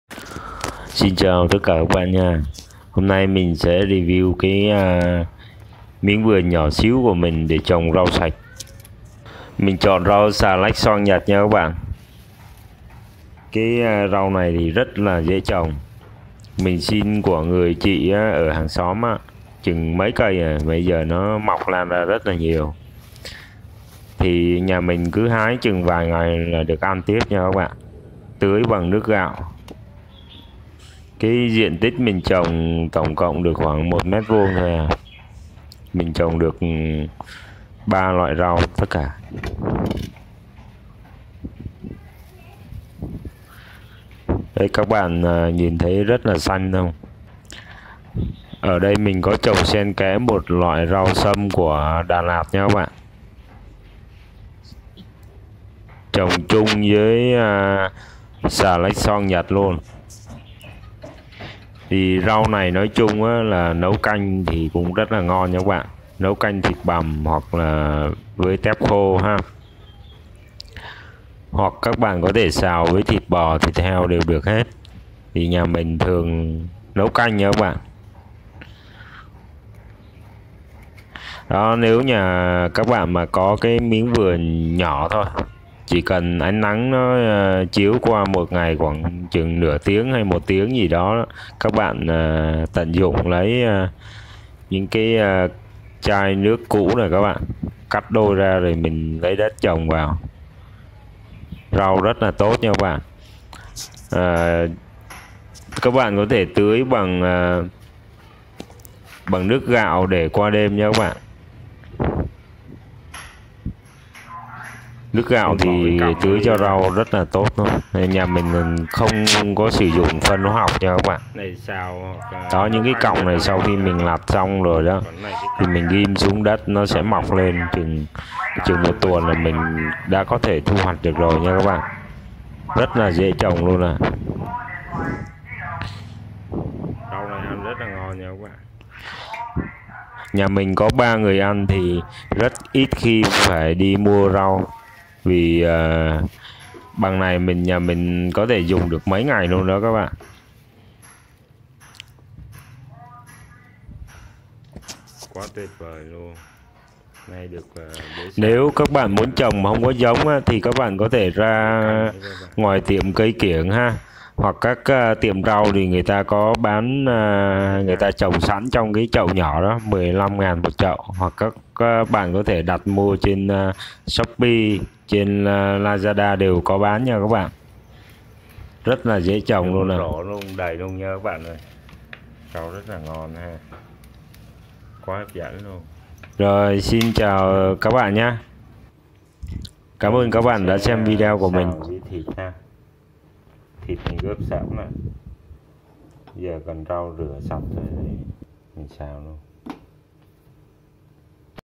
trồng rau sạch ở nhà, 1 lần trồng ăn một đời | grow vegetables on the terrace in vietnam
Đây là rau xà lách xoong nhật nha các bạn, các bạn có thể tận dụng mọi khoảnh đất có thể để trồng nó. Dùng nước vo gạo và các loại rác thực phẩm để làm phân bón cho cây nha các bạn. Chúc các bạn thành công !